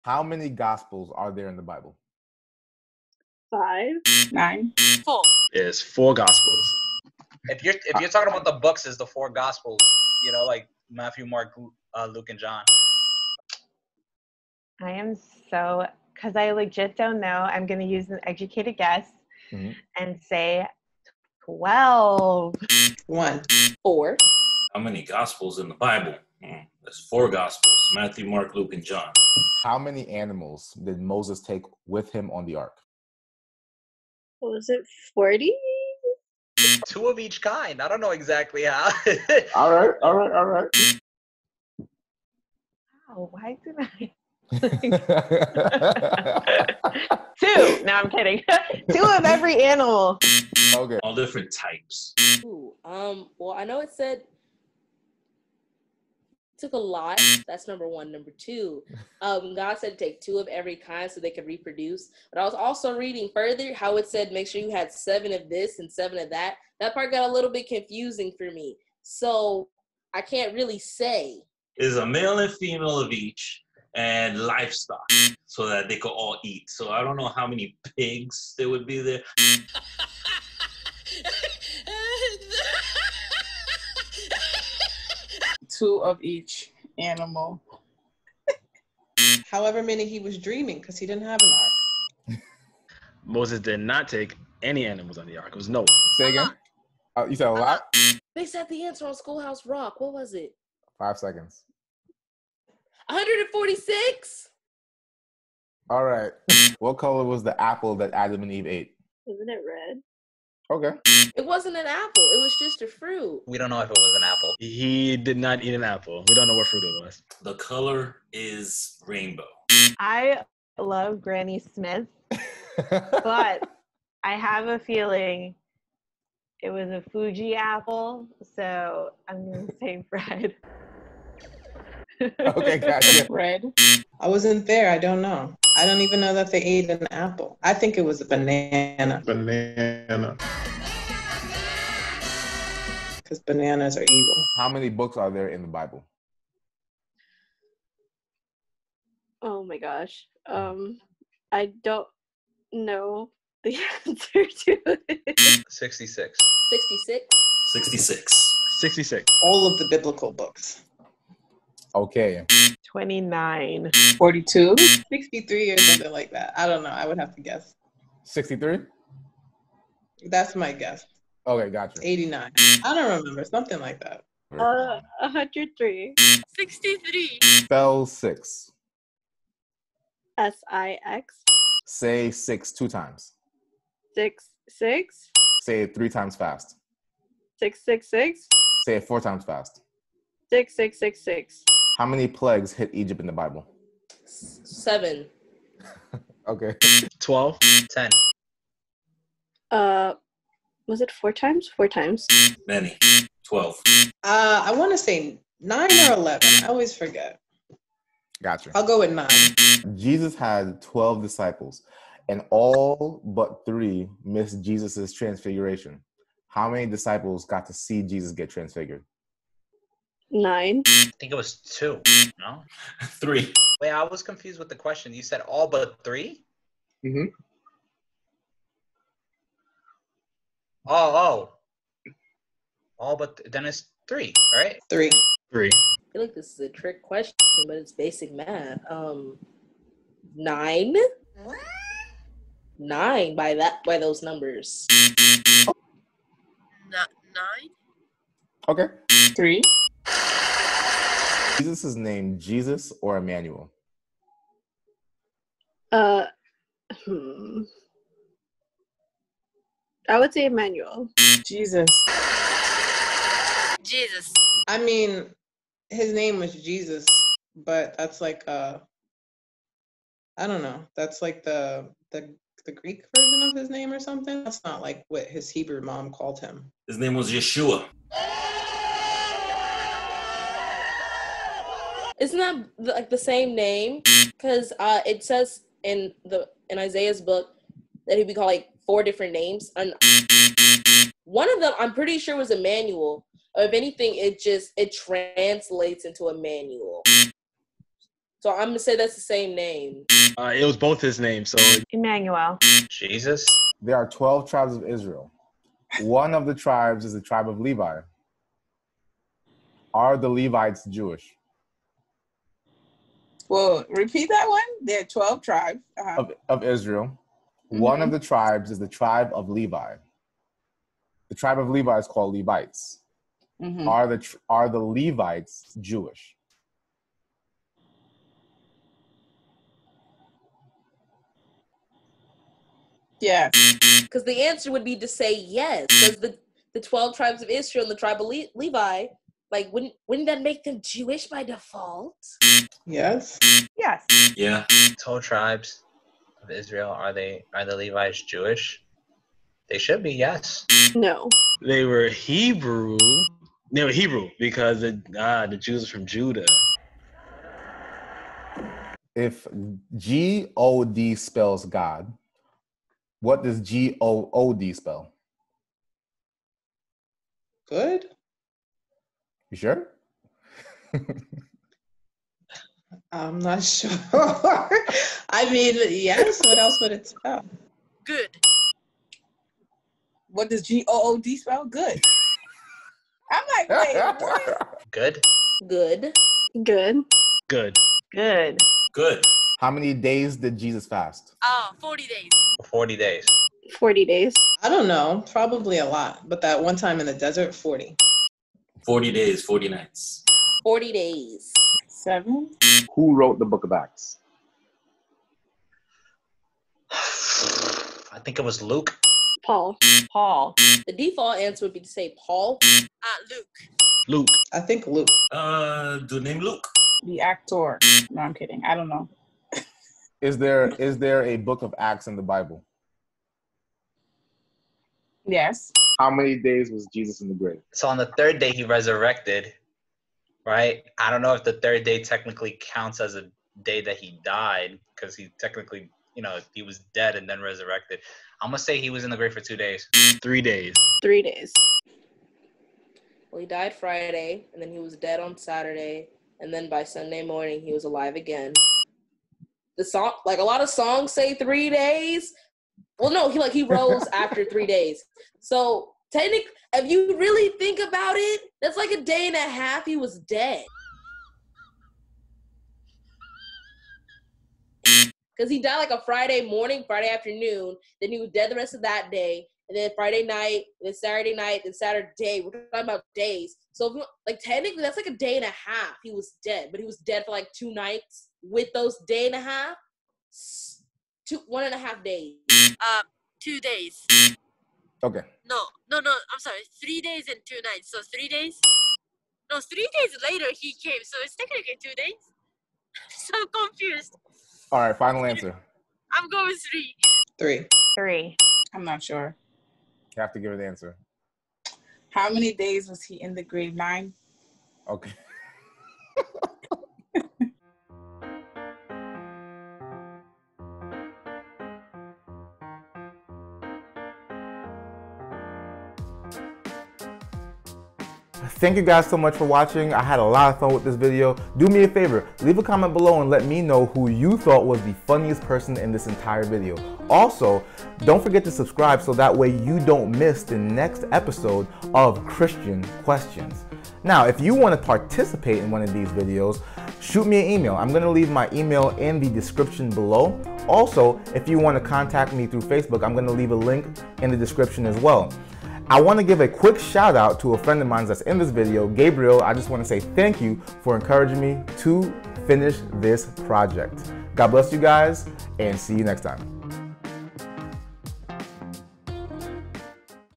Christian Questions S1: E1: Asking christians biblical questions you'd think they'd know.
*If G-O-D spells God what does G-O-O-D spell lol.* (everyone got this question right)
Special thank you to everyone that participated in this video, August was a discouraging month but working on this video made me laugh the whole way through.

Credit for the sound effects